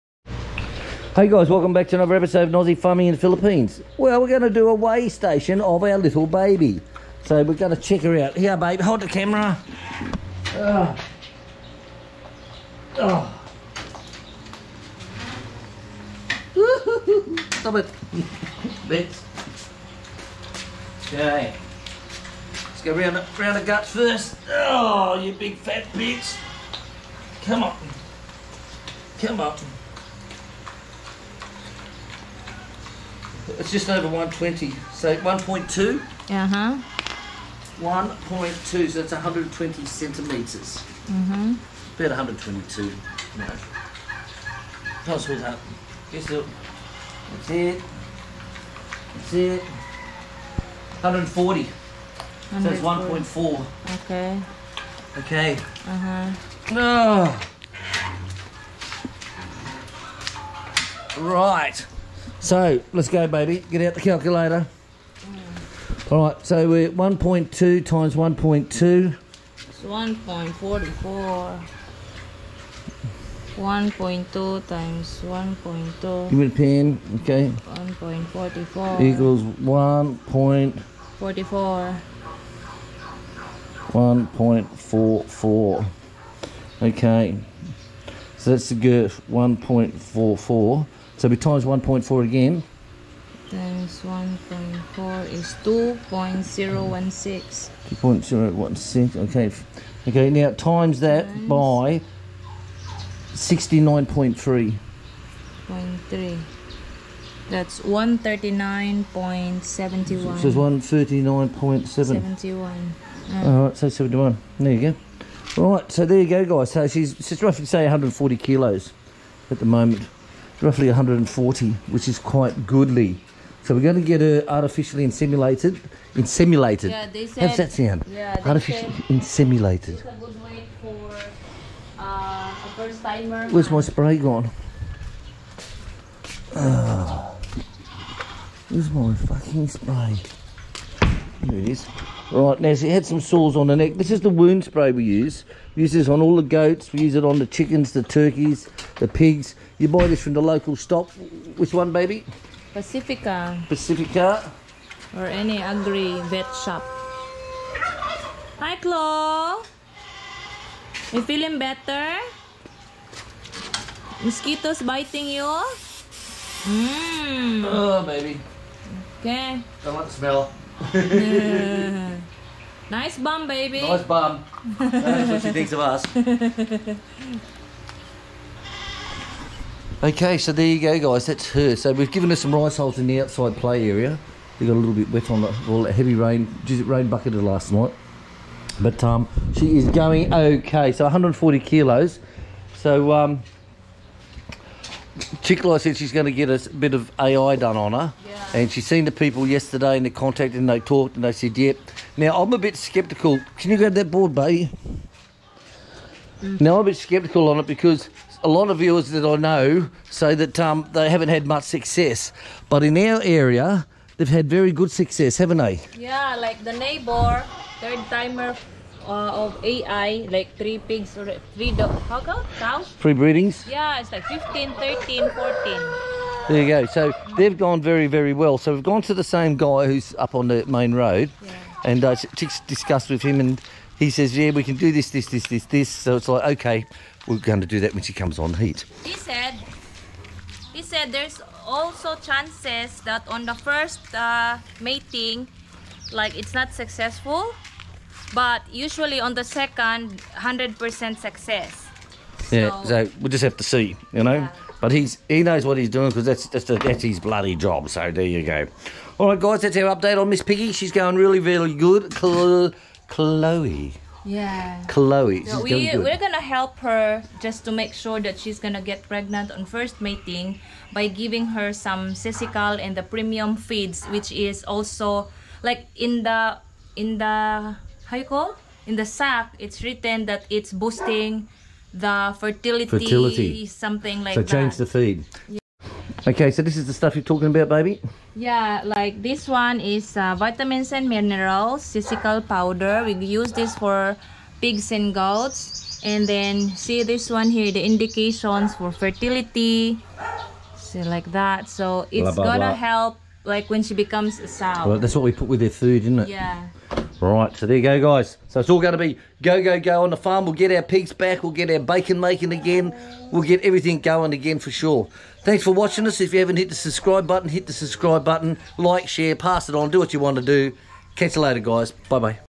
HOW TO - Weigh a Pig Without Scale in Kg - Pig Live weight
Today we will show you all HOW TO - Weigh a Pig Without Scale in Kg. weighing pigs without scales is very easy to do , with this easy way to measure a pig way to find the live weight

METRIC VERSION - Weight of your pig in KILOGRAMMES
Square the Heart Girth to get the Girth Result.
Now Multiply the Girth Result by the Length and MULTIPLY by 69.3.
You now have the weight of your pig in Kg.

William Wilcox
8105 Panabo City
Davao del Norte PHILIPPINE